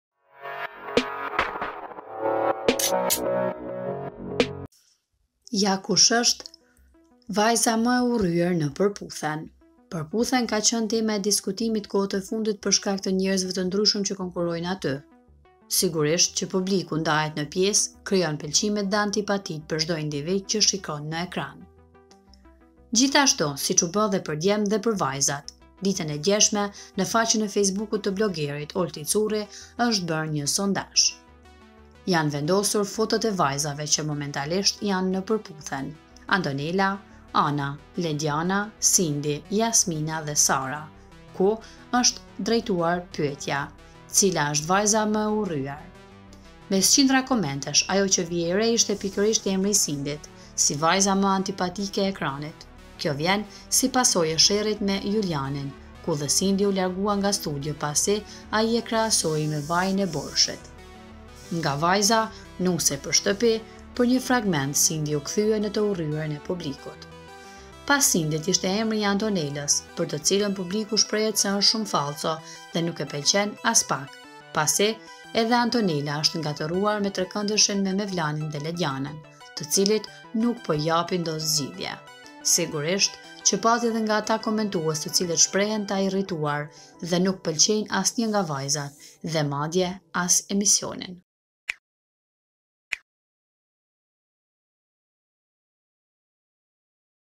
Vajza me e urryer e publikut eshte ....!
Sigurisht qe publiku ndahet ne pjese krijon pelqimet dhe antipatite per cdo individ qe shikon ne ekran. Diten e sotme ne faqen e Facebook-ut te blogerit Olti Curri eshte bere nje sondazh.
Jane vendosur fotot e vajzave qe momentalisht jane ne Perputhen: Antonela, Ana, Lediana, Sindi, Jasmin dhe Sara ku eshte drejtuar pyeta: Cila eshte vajza me e urryer.
Mes qindra komentesh ajo qe vihej re ishte pikerisht emri i Sindit si vajza me antipatike e ekranit. Kjo vjen si pasoje e sherrit me Julianin ku dhe Sindi u largua nga studio pasi ai e krahasoi me vajin e Borshit. Nga vajza “nuse per shtepi” per nje fragment Sindi u kthye ne te urryeren e publikut.